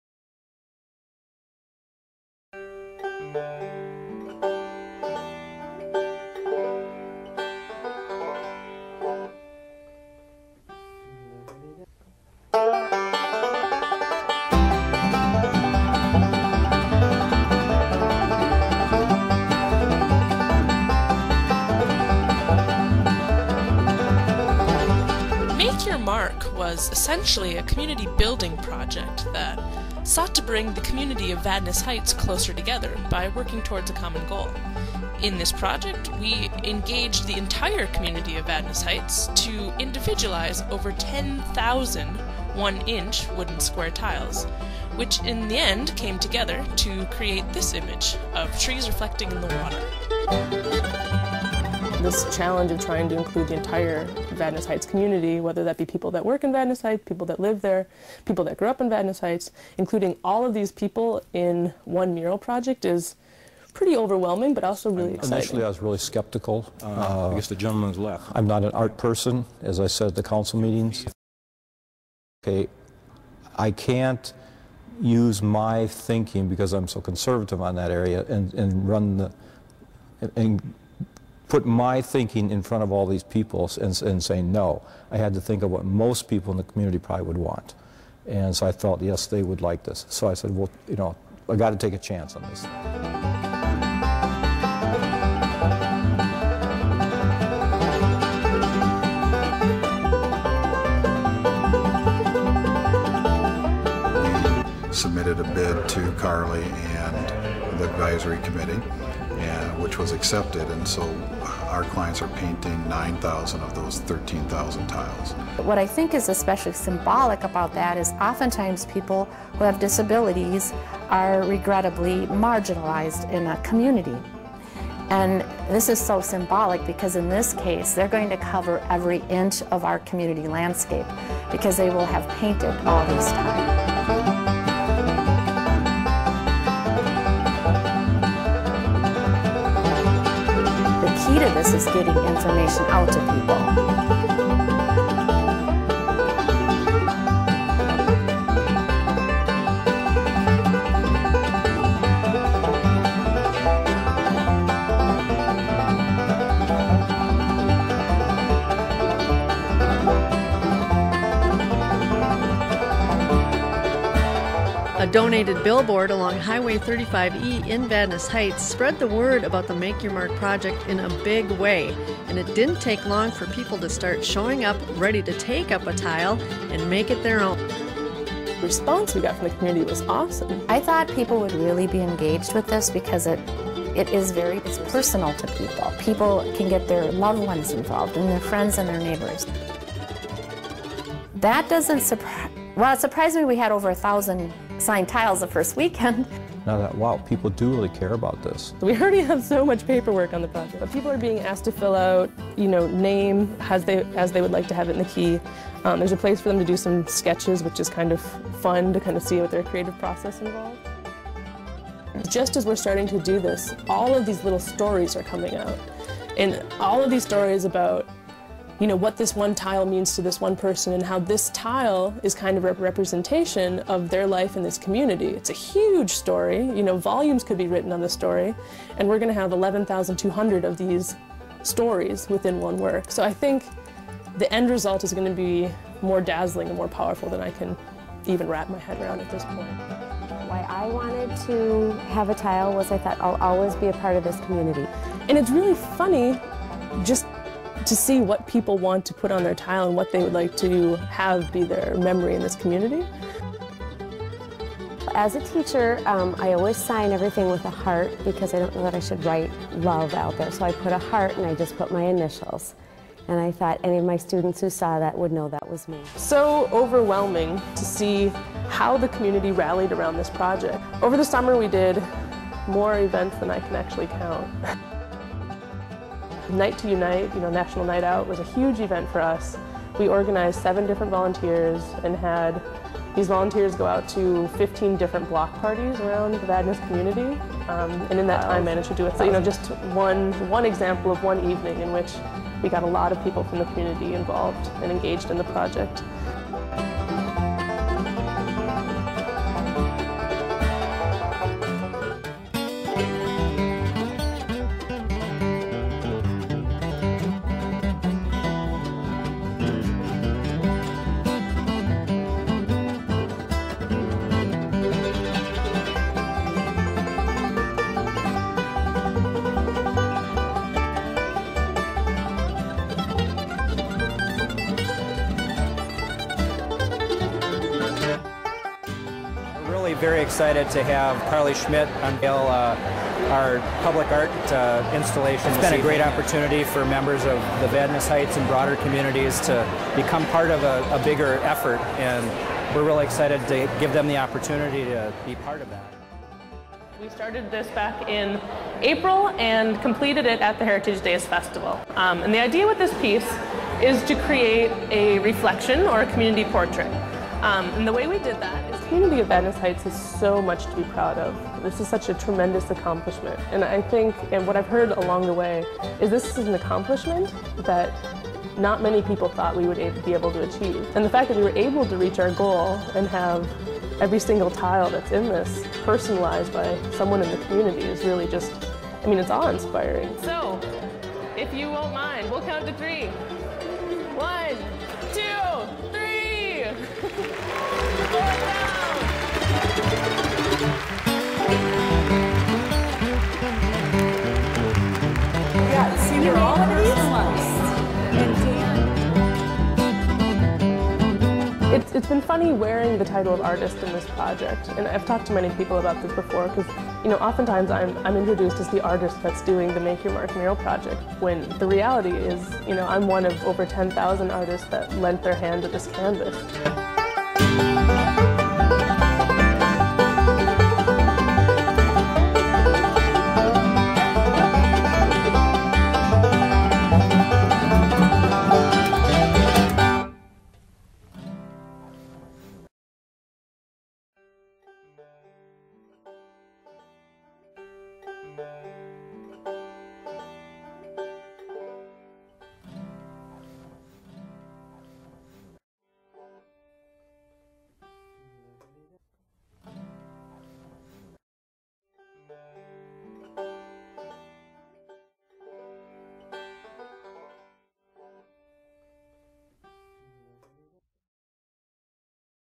Make [Y]Our Mark, Vadnais Heights MN 2011
A collaborative community art project that engaged an entire community in the creation of a community portrait.This project worked to create a collective human experience that encourages participants to think beyond their individual mindset. Members of the Vadnais Heights community personalized 11,424 one-inch wood tiles that came together to create two 4 x 10 foot mosaic panels. Participants were invited to embellish their one-inch pixel with a similar colored paint, so that close-up you can see the individual contributions, but from a distance the individual details fade out as a larger image of the community emerges. The concept mirrors an individual's place in a community and the whole is greater than the sum of its parts. This mosaic documents a unique time in this community when so many people worked together for a common good.